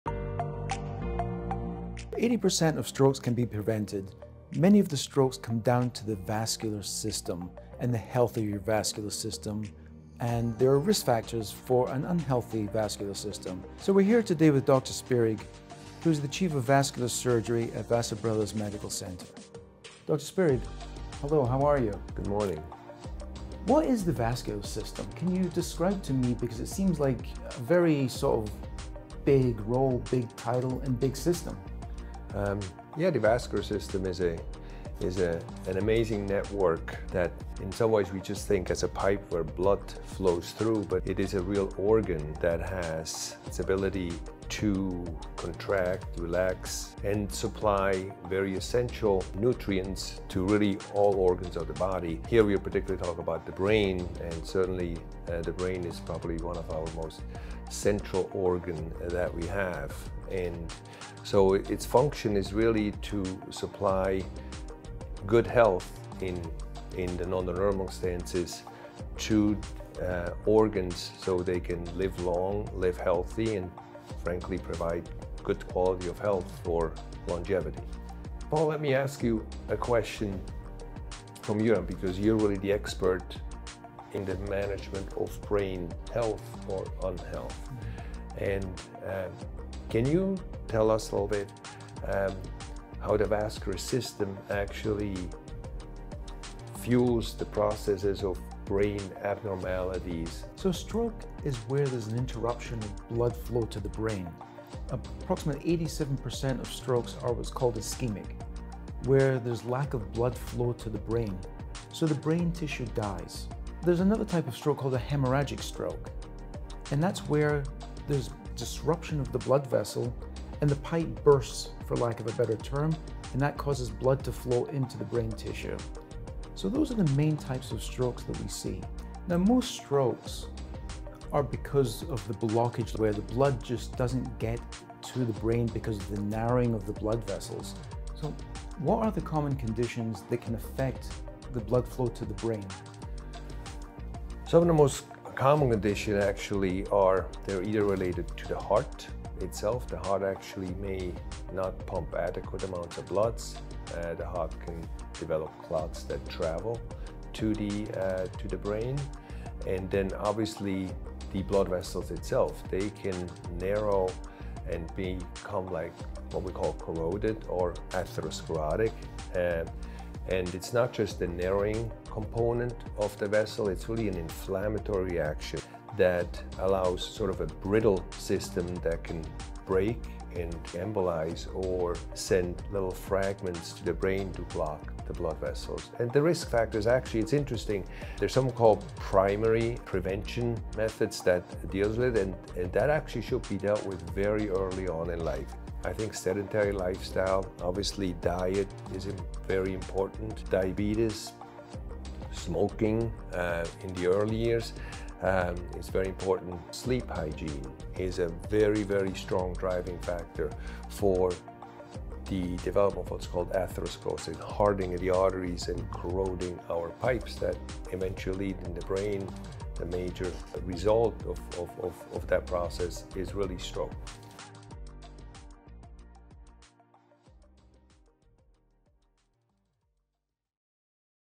Find Out Why Vascular Health is Critical for Brain Health
Did you know about 80% of strokes can be prevented? It all comes down to your vascular health. Your vascular system works hard for you. But do you know what it does and how critical its health is to stroke prevention? Vascular surgeon Dr. Andreas Spirig and neurologist Dr. Paul Wright explain the crucial role of the vascular system in lowering your risk of stroke, increasing your longevity and boosting brain health. Watch now!

Check out this related content – How to keep your vascular system healthy and prevent strokes